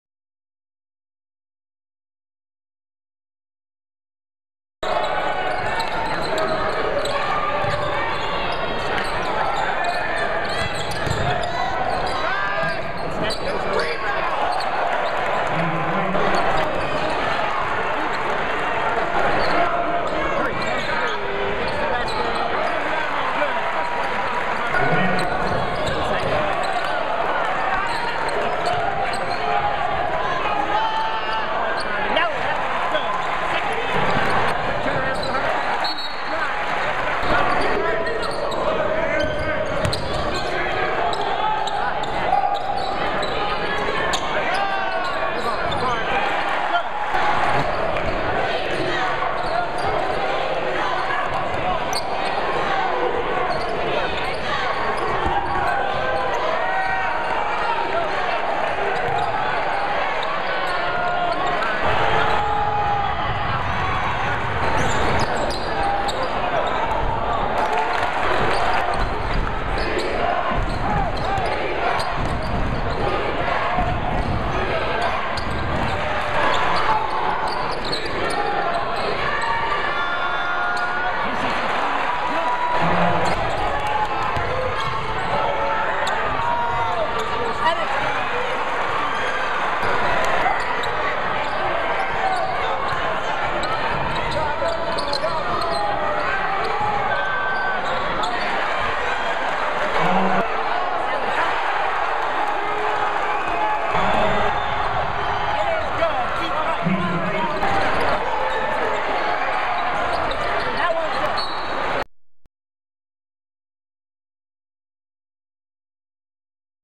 Highlights: No. 21/22 Harvard Men's Basketball at Fordham
The No. 21/22 Harvard men's basketball team fell on the road at Fordham, 60-54, Jan. 3, 2012.